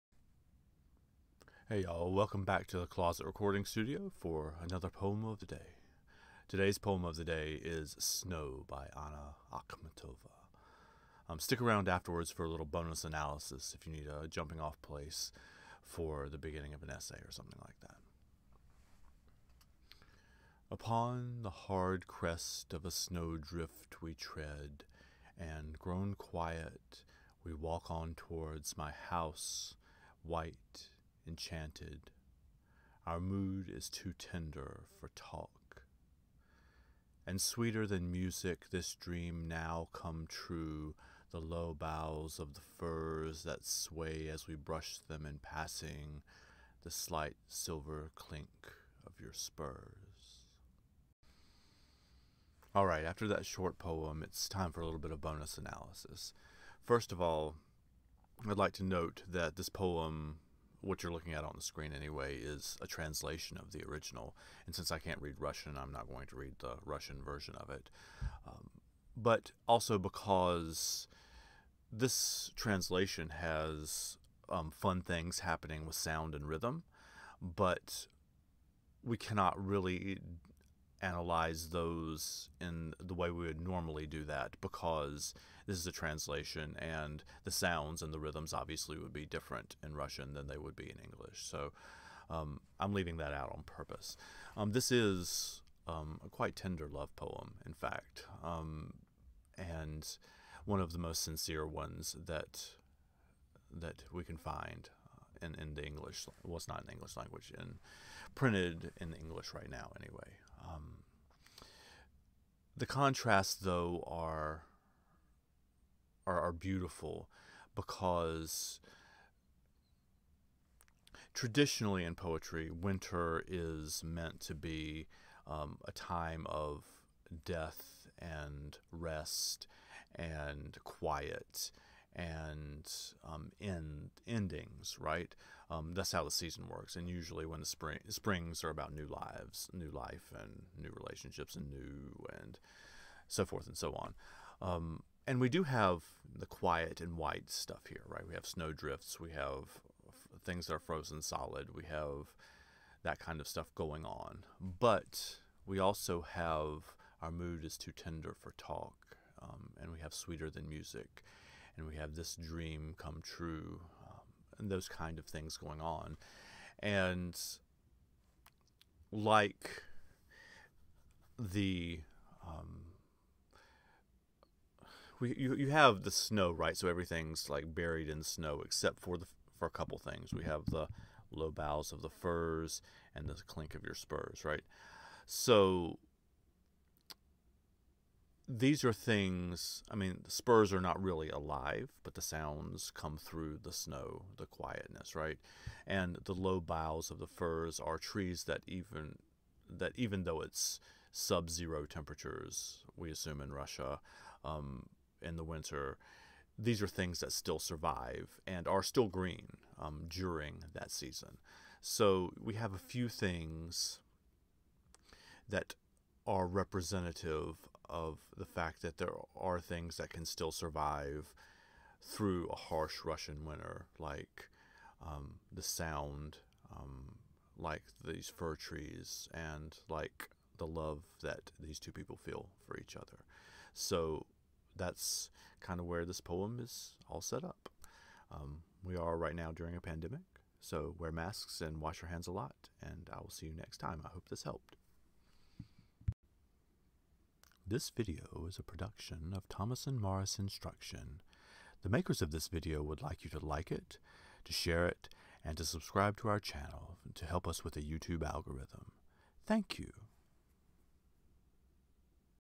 "Snow" (Anna Akhmatova) - Poetry Read-Aloud and Reaction/Analysis
A read-aloud -- and then a veteran English teacher gives some commentary on structure, form, figurative language, etc. For the purposes of essay help!